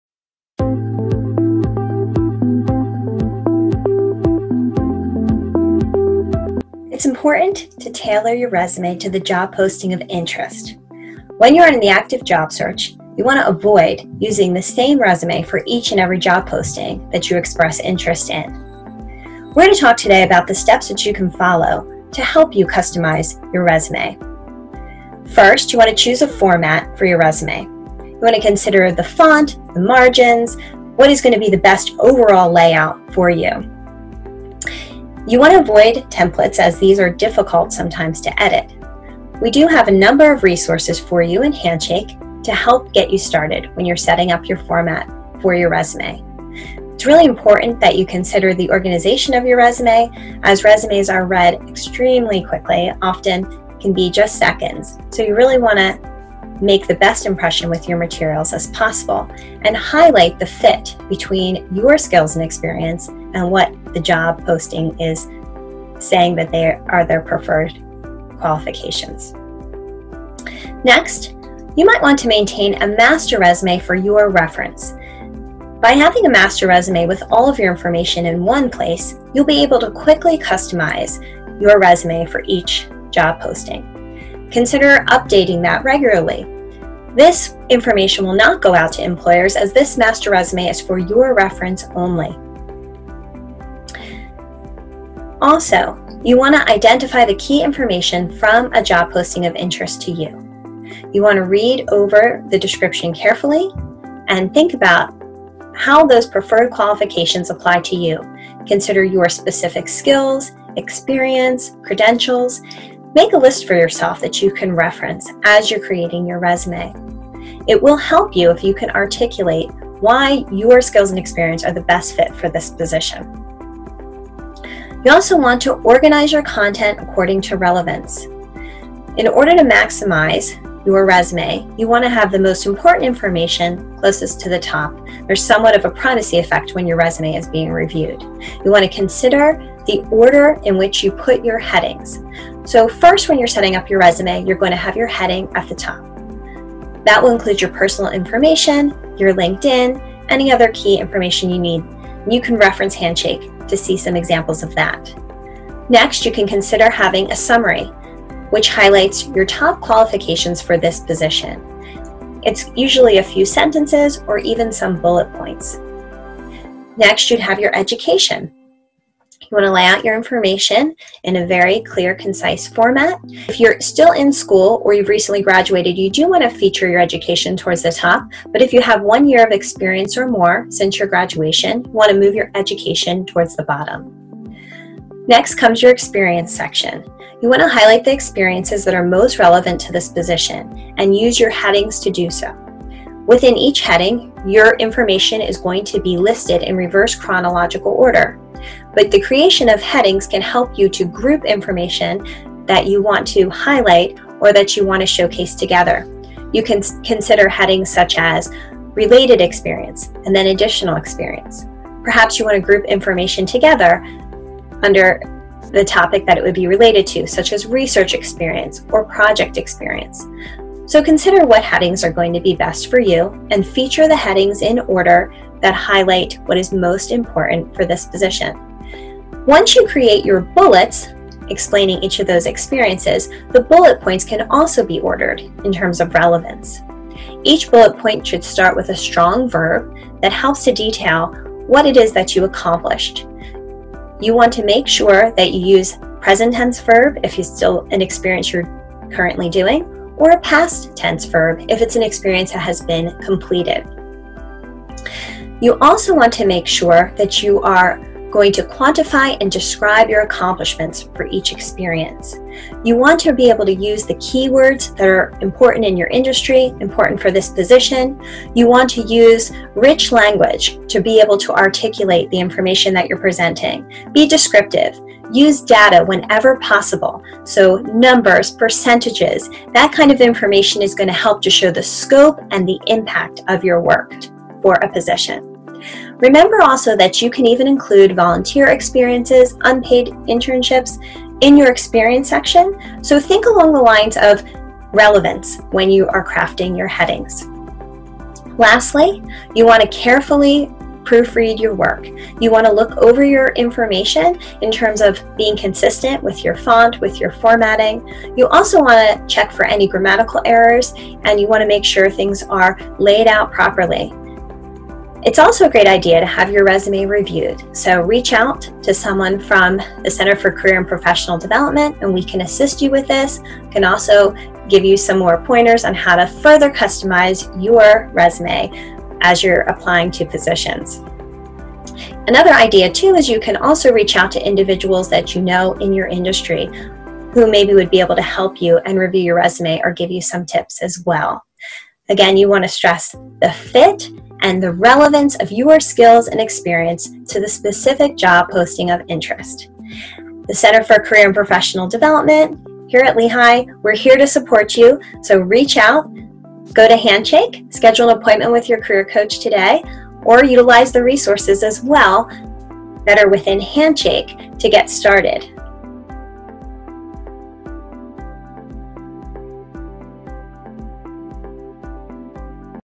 Tailor Your Resume To A Specific Position
Tailoring your resume to a specific position can help you stand out as an applicant. This video will show you how to tailor your resume to a job posting of interest to show the relevance between your qualifications and those of the job description.